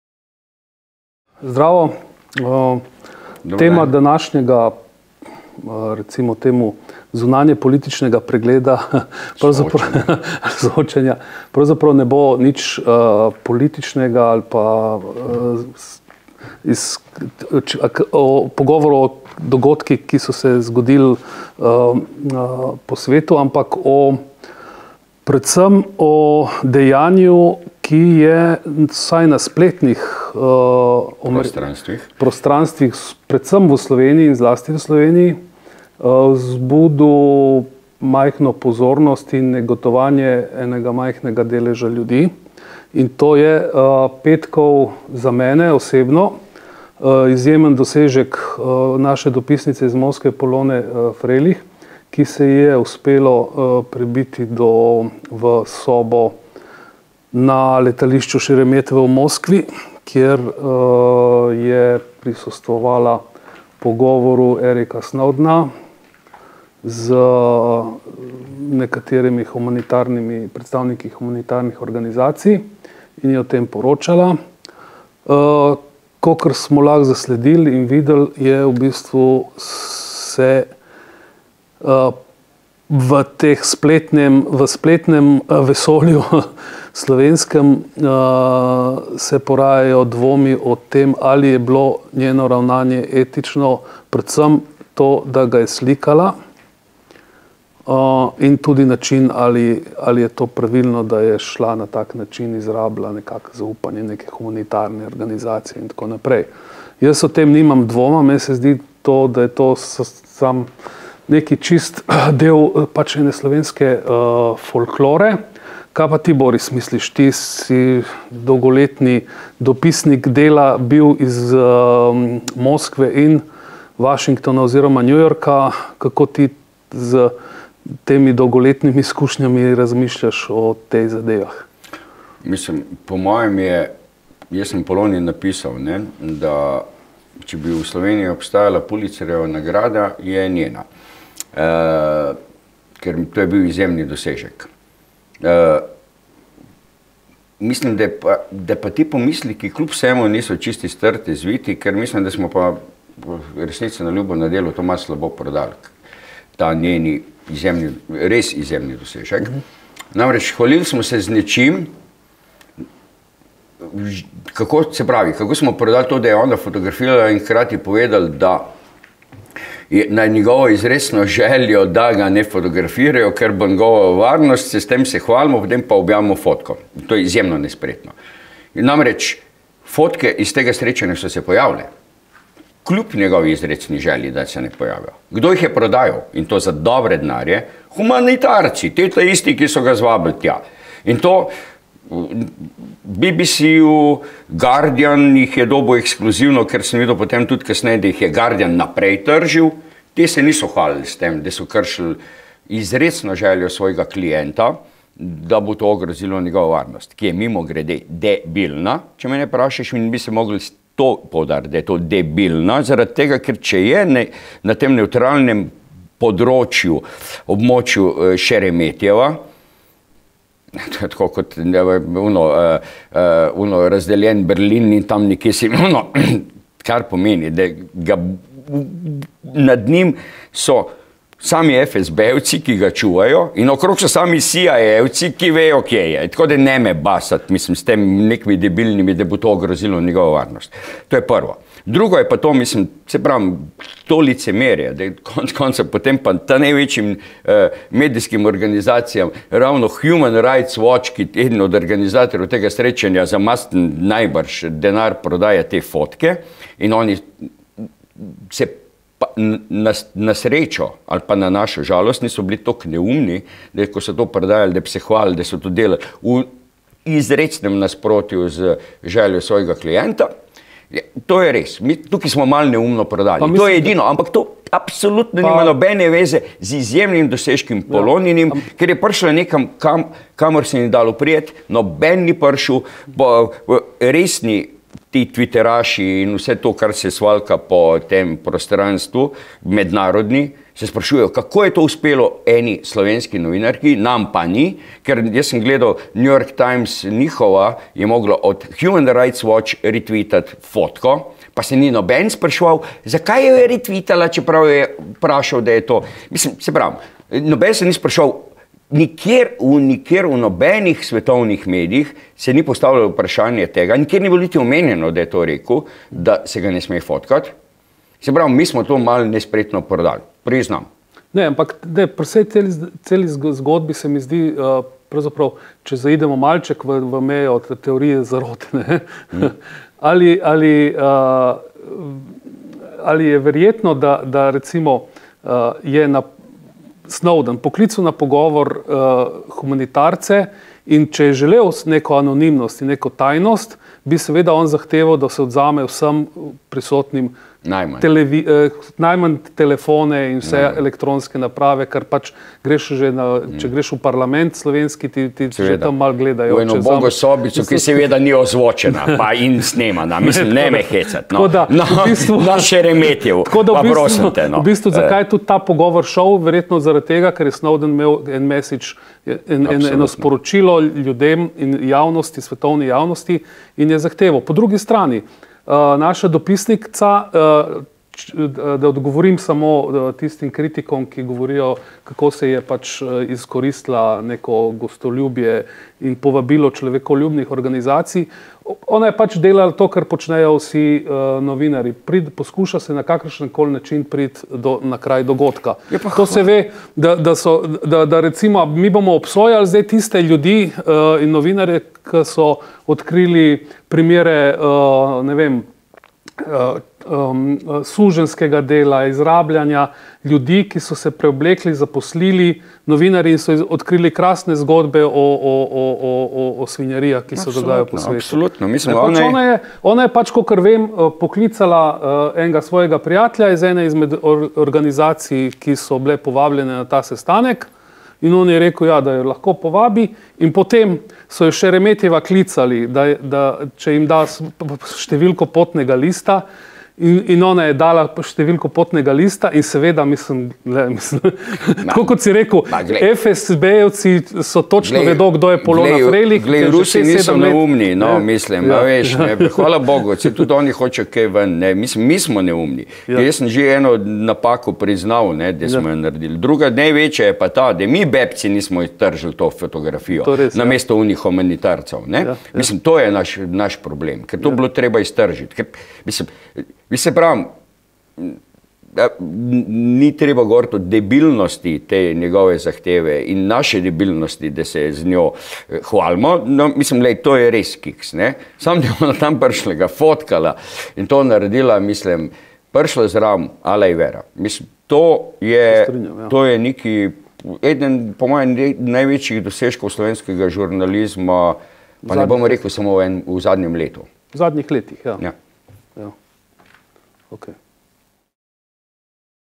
Po srečanju s Snowdnom: hipokrizija in etika
Dopisnici Dela je uspel izjemen dosežek, ki pa je v slovenskem spletnem vesolju zbudil tudi negodovanje. Pa premislimo, zakaj?

Dopisnica Dela iz Moskve Polona Frelih je minuli petek sedela v isti letališki sobi kot znani ameriški žvižgač Edward Snowden. Prišla je, kamor ni skoraj nihče, in o tem tudi poročala.

Urednik zunanjepolitične redakcije Veso Stojanov ter nekdanji dopisnik iz Moskve in New Yorka Boris Čibej se strinjata, da ji je uspel izjemen dosežek.

V slovenskem spletnem prostranstvu pa so se pojavili dvomi: ali je bilo njeno ravnaje etično, je zlorabila Snowdnovo zaupanje in ogrozilo njegovo varnost?

Sogovornika se sprašujeta, ali so morda takšni pomisleki zgolj del slovenske folklore. V odgovor med drugim očitek o licemerstvu usmerita v drugo smer, k nevladnim organizacijam, in navedeta, da so se resni tviteraši po svetu raje spraševali, kako je uspelo njej, njim pa ne.